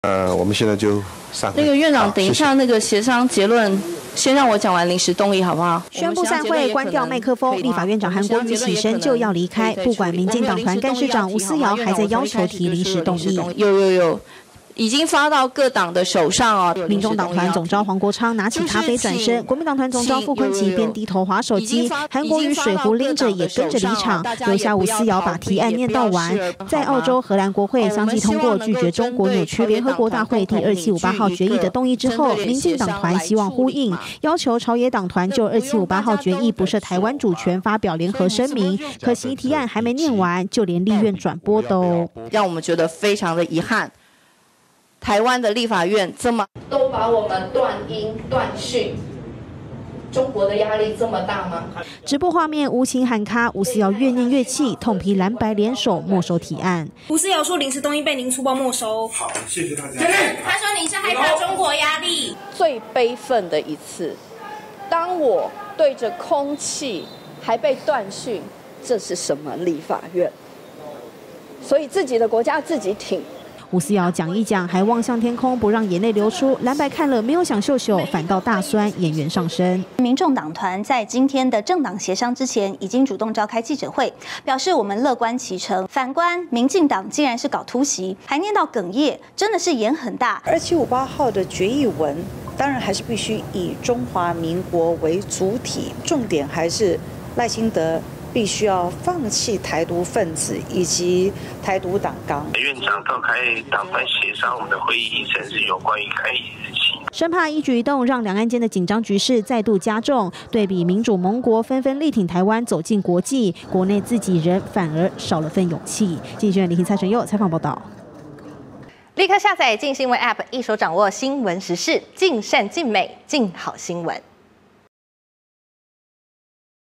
提「2758決議不涉台」 吳思瑤淚訴：韓國瑜閃人｜鏡新聞
澳洲、荷蘭國會日前相繼通過「拒絕中國扭曲聯合國第2758號決議」的挺台動議，民主國家紛紛力挺台灣走向國際。立法院民進黨團今天（18）在黨團協商中，想以臨時動議方式提出呼應文，但立法院長韓國瑜事後卻宣布散會，藍白黨團幹部更直接離場，讓民進黨立院黨團幹事長吳思瑤哽咽痛批，「這是最悲憤的一次朝野協商」。